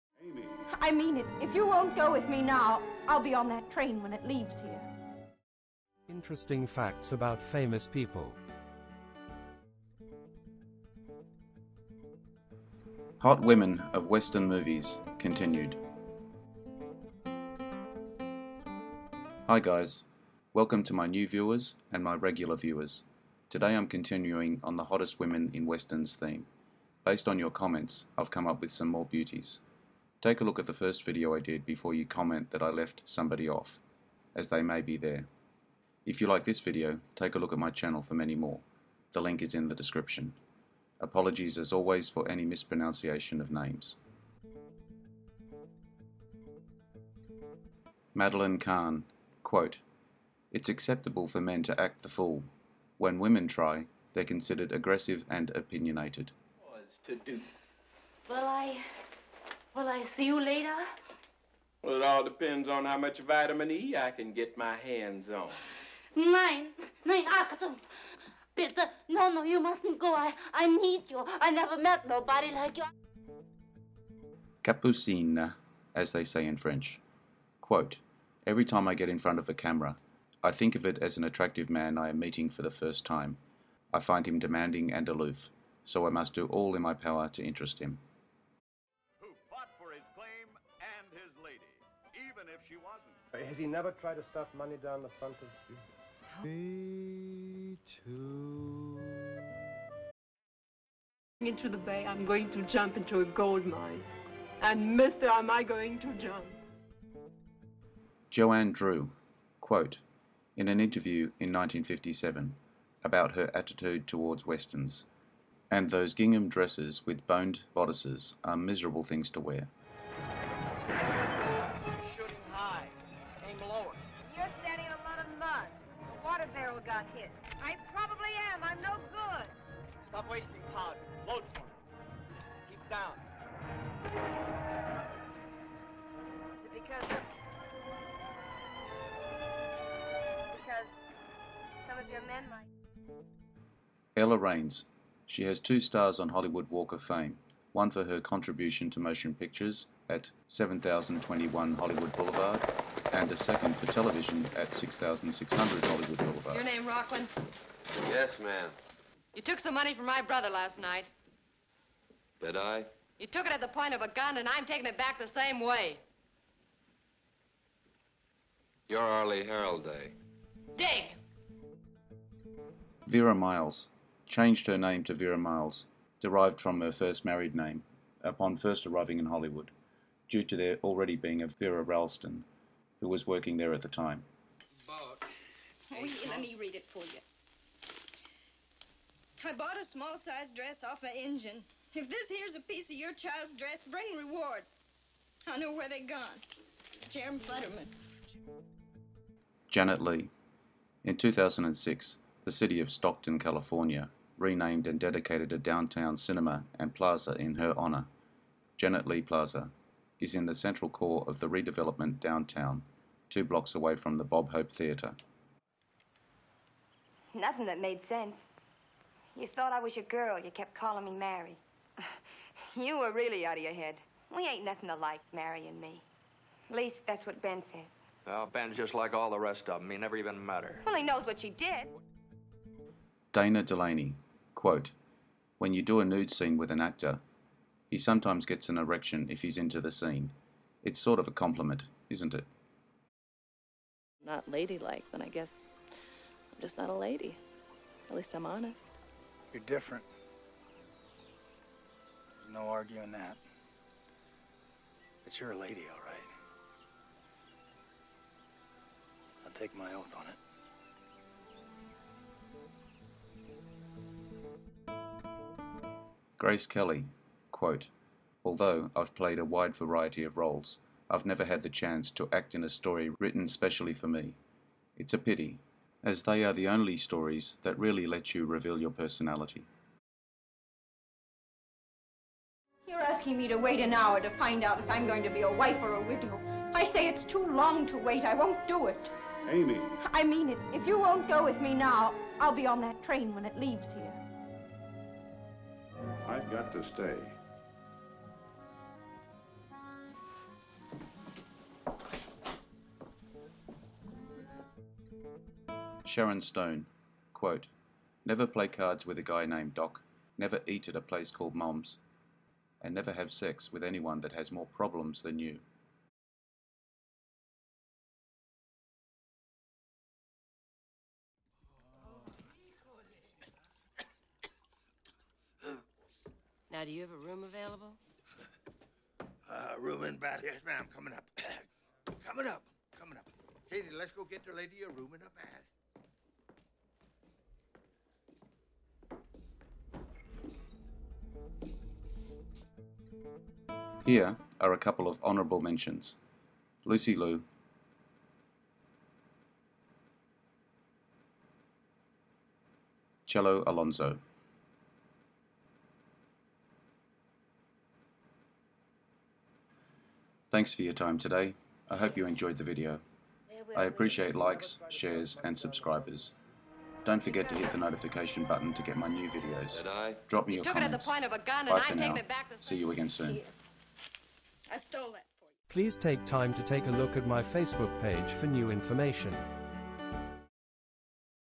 Hot Women of Western Movies
Hi guys, welcome to new viewers and my regular viewers. Today I'm continuing on the Hottest women in Westerns theme. Based on your comments I’ve come up with some more beauties. Take a look at the first video I did before you comment that I left someone off, as they may be there.